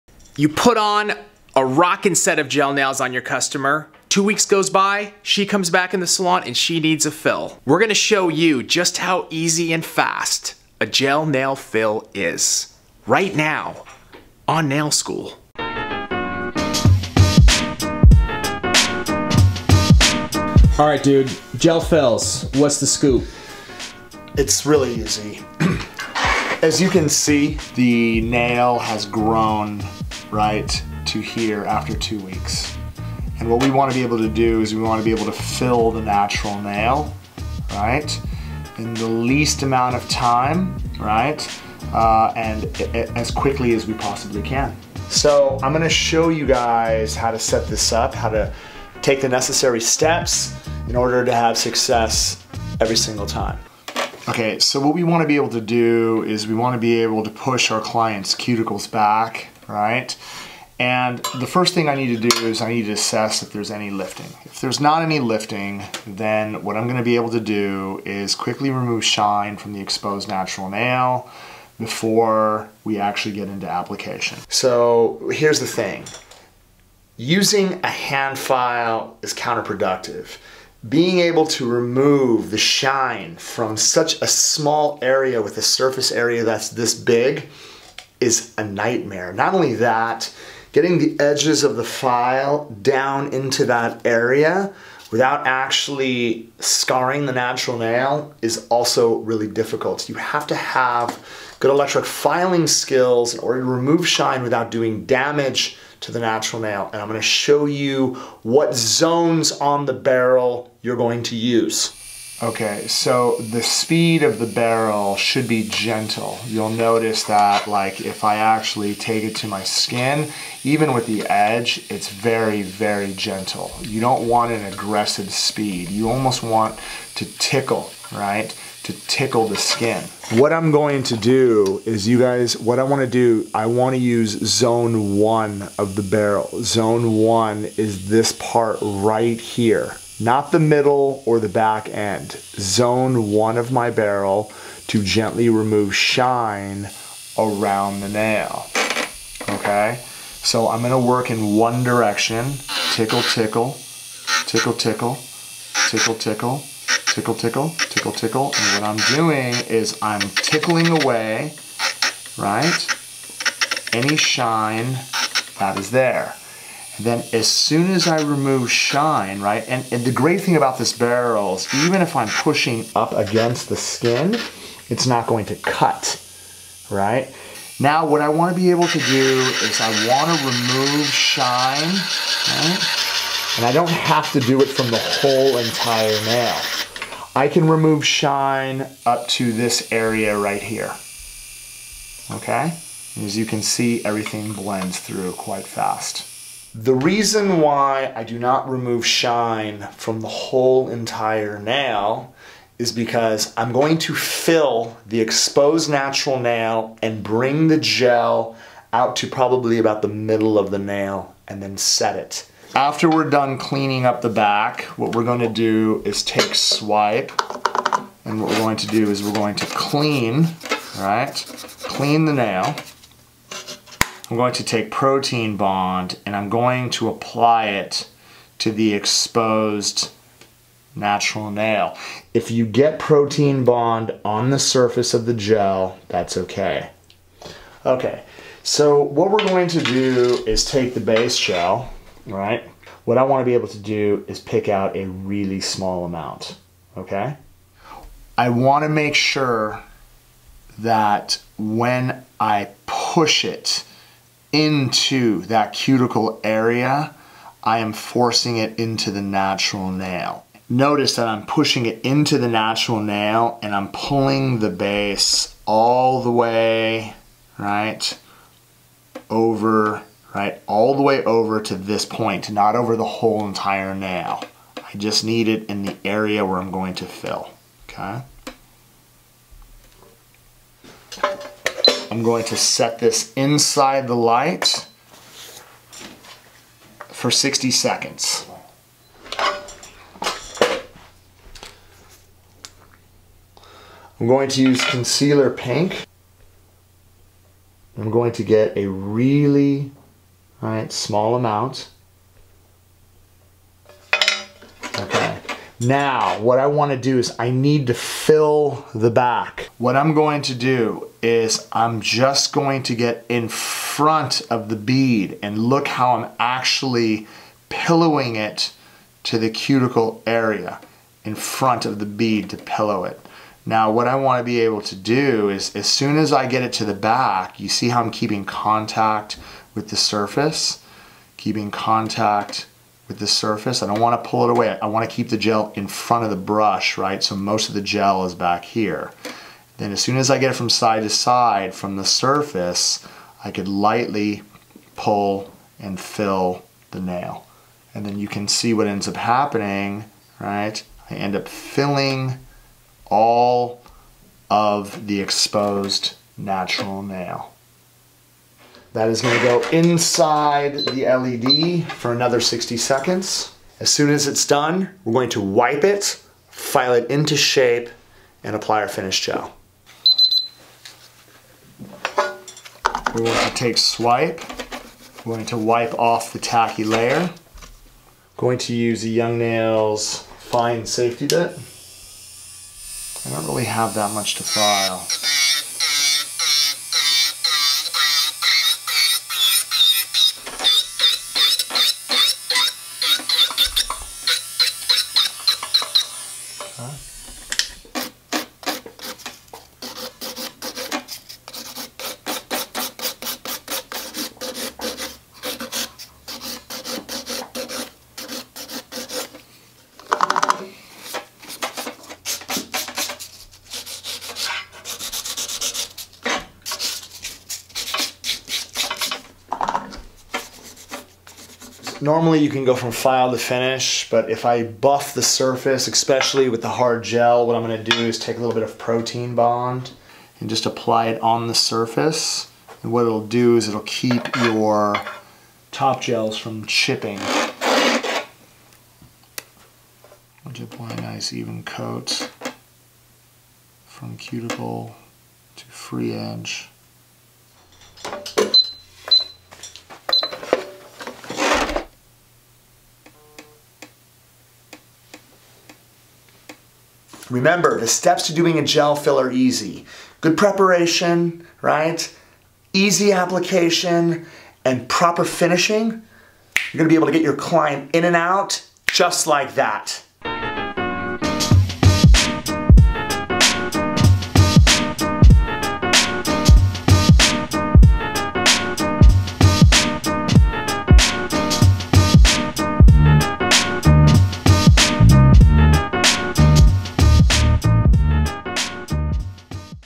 YN NAIL SCHOOL - DOING A GEL FILL AFTER 2 WEEKS GROWTH
Greg takes you through the fundamentals of doing a Gel Fill after your client comes back after 2 weeks.